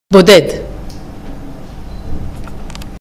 ISL: בודד Lonely
Sign for "Lonely" in ISL (Israeli Sign Language)
בודד: בשפת סימנים ישראלית
Pronounced: Boded
Signer: Anna